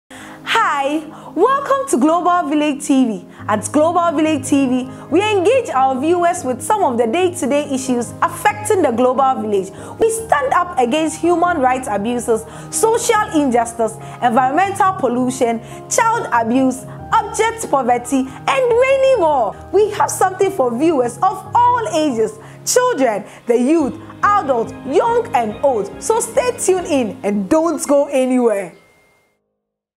HI WELCOME TO GLOBAL VILLAGE TV
AT Global Village TV we deliberate on some of the day to day issues affecting our common planet-- poverty, unequal distribution of wealth, healthcare challenges, climate change etc.
GLOBAL VILLAGE TV-- -where the people of the world meet to share ideas.